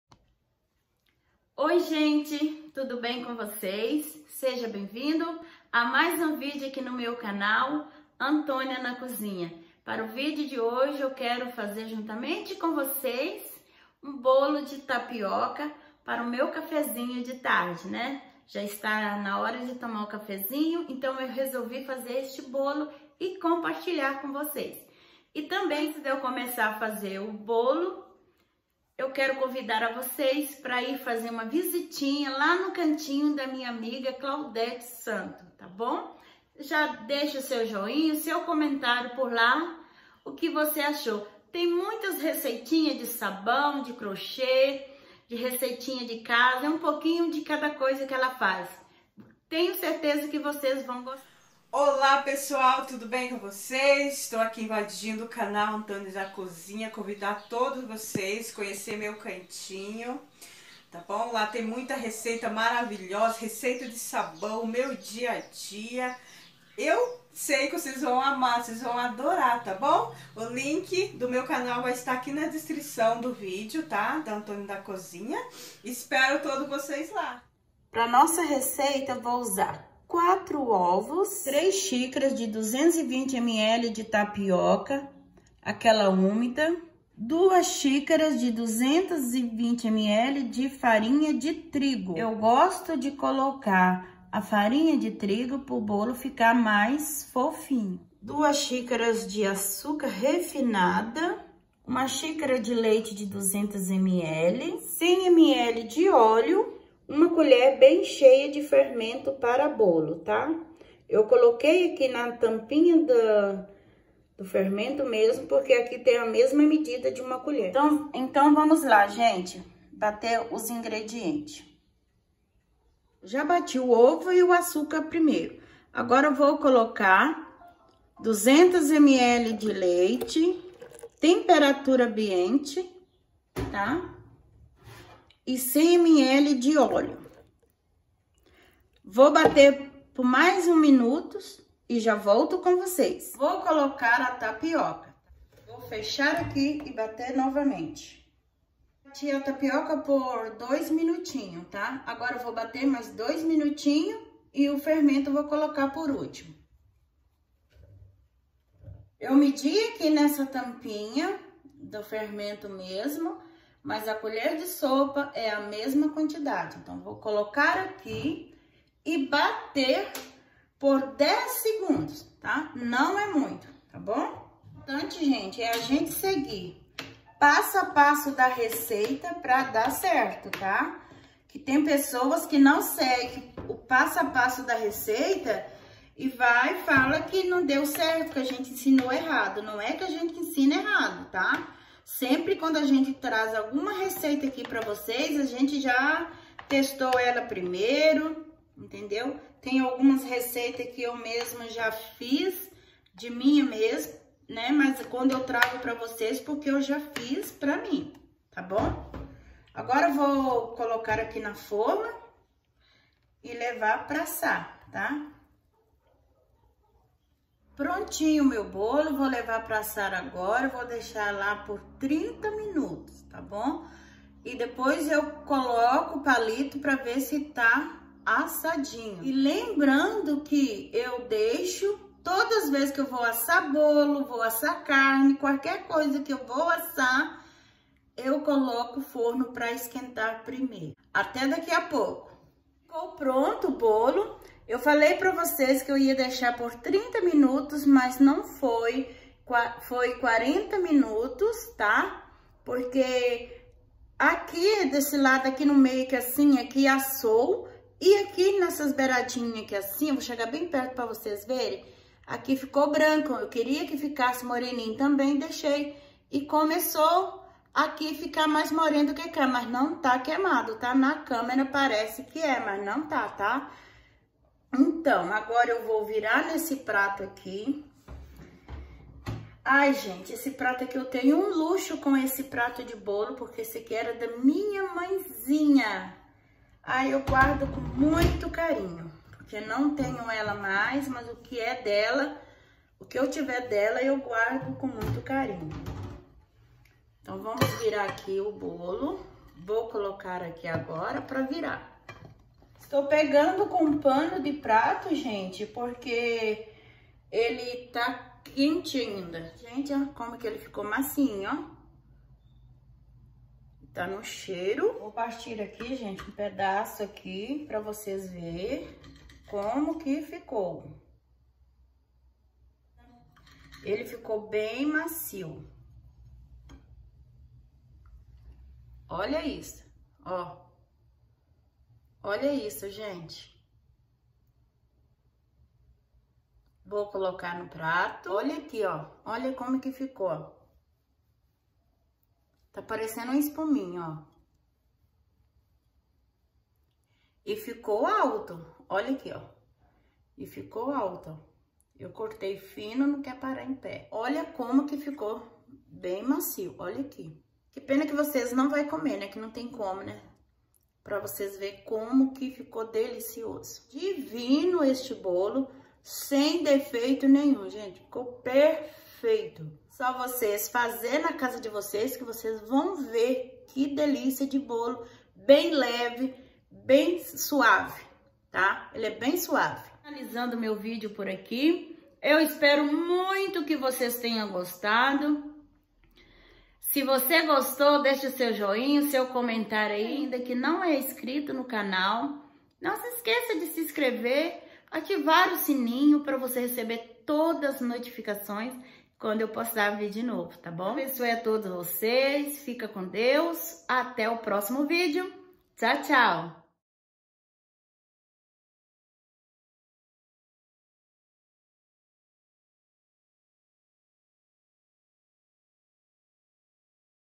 Bolo de tapioca Fofinho
Bolo de tapioca

- 3 xícaras de farinha de de tapioca úmida de 220 ml
-2 xícaras de farinha de trigo
- 4 ovos inteiro
-1 colher de sopa de fermento biológico
- 2 xícaras de açúcar
-100 ml de óleo
-200 ml de leite temperatura ambiente